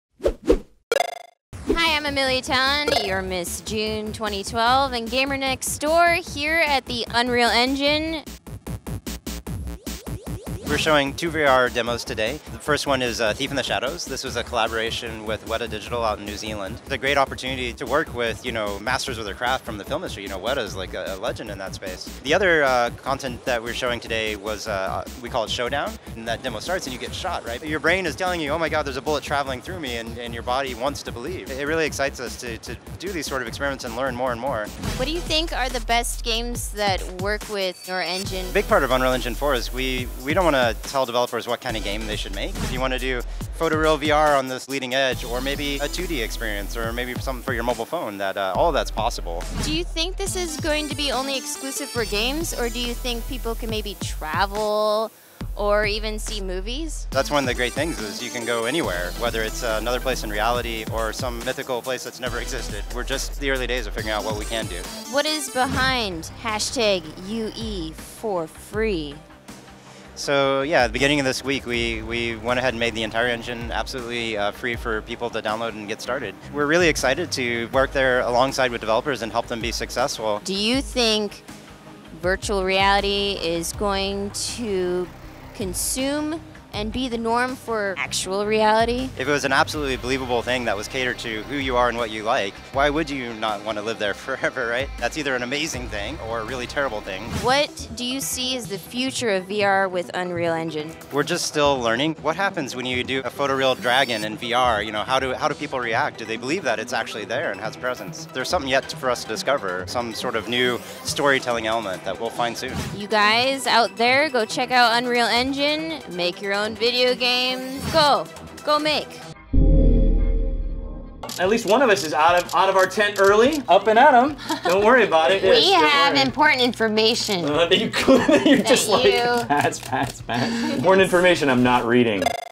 Amelia Talon Gets Virtual with The Unreal Engine - Game Developers Conference
At the 2015 Game Developers Conference, Amelia interviews Ray Davis, who peels the curtain back on Unreal Engine -- which allows programmers to get their hands on next-gen virtual-reality tools, free of charge. Watch as she gets an exclusive demo and finds out what the future of virtual reality holds with Unreal Engine. Game on!